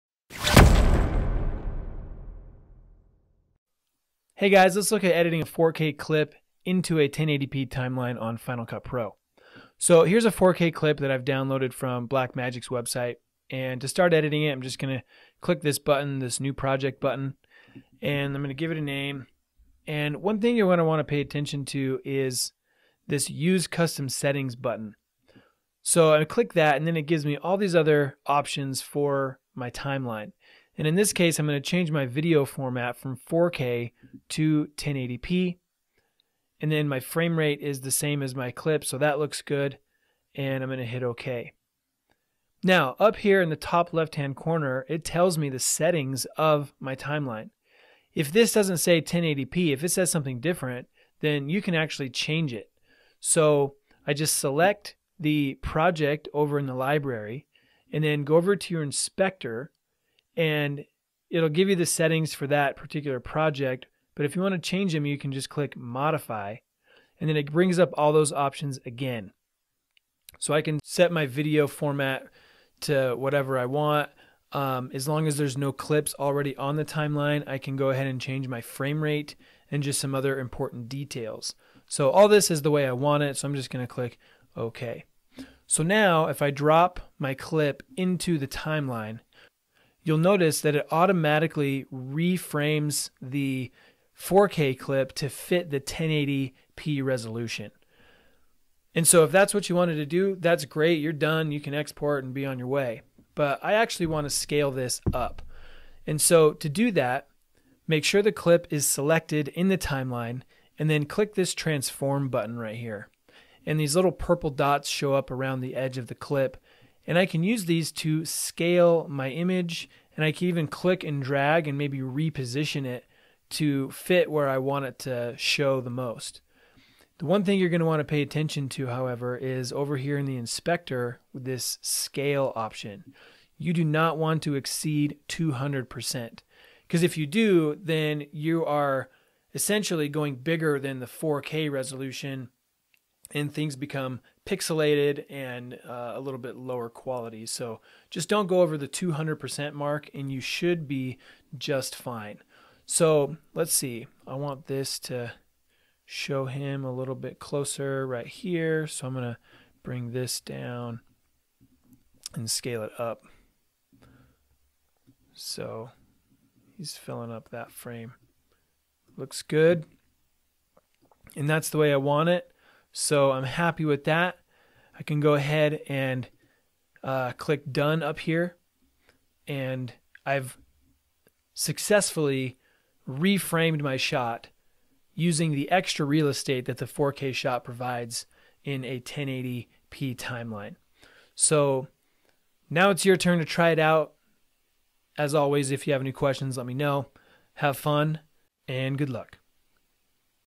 Editing 4K Clips in a 1080p Timeline in Final Cut Pro
Shooting in 4K is nothing new these days. However, you may still choose to edit at 1080p. Here's a basic tutorial on how to do that in Final Cut Pro.